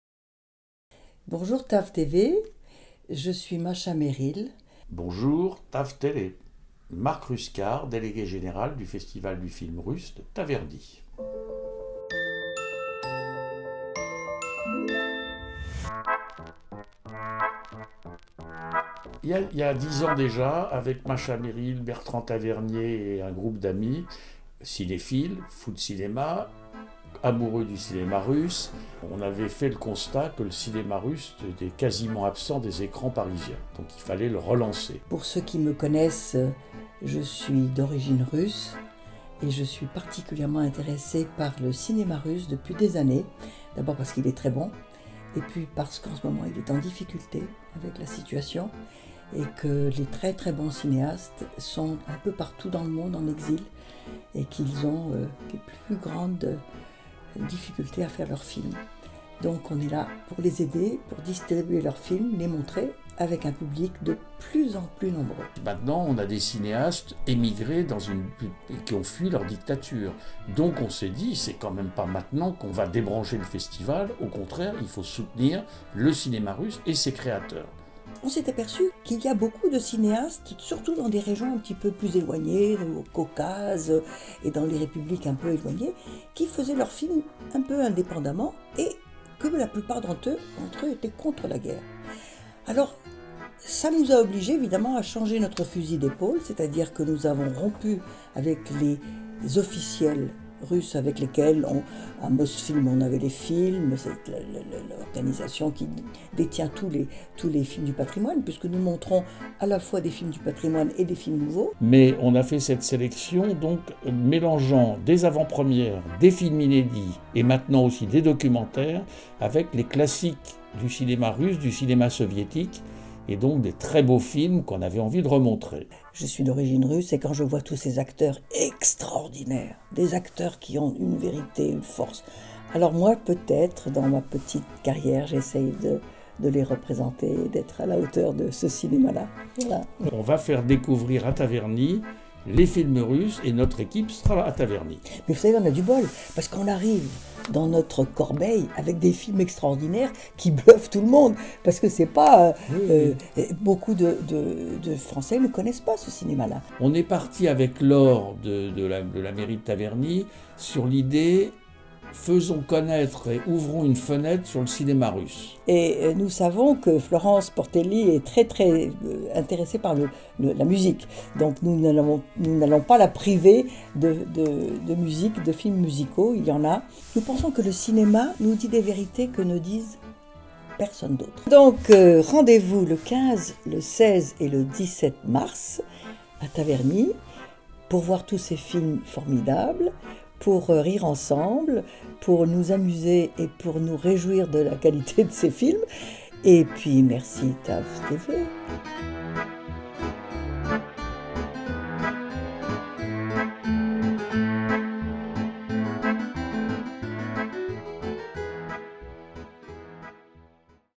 ANNONCE - FESTIVAL DU FILM RUSSE À TAVERNY
TAVERNY accueille le festival du cinéma russe indépendant mi - Mars.

La talentueuse Macha Méril, qui en est la présidente nous le présente.